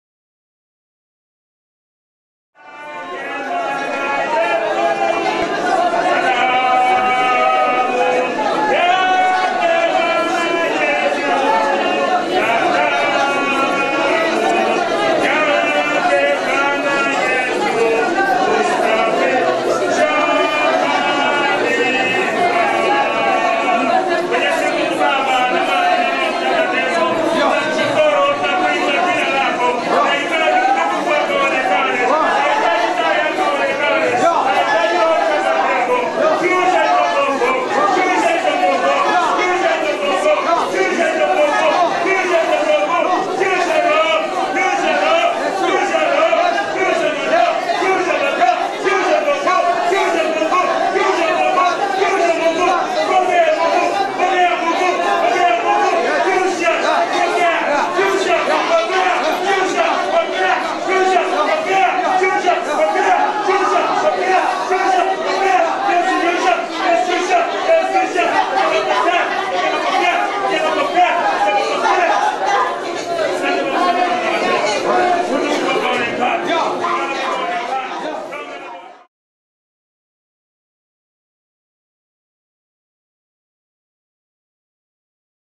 African Intercession and Deliverance
Intercession, deliverance and healing are also activities of the Church that Jesus Christ founded.  Corportate church prayer is not an option.

Keep in mind... these churches in Africa meet all day... with 2 four hour sessions, without padded pews, (only benches), without carpeted floors (only dried mud), and without electricity!  Ask your Pastor to try it for just one sunday service... and just pretend that you are back in the Book of Acts Church!  Amen.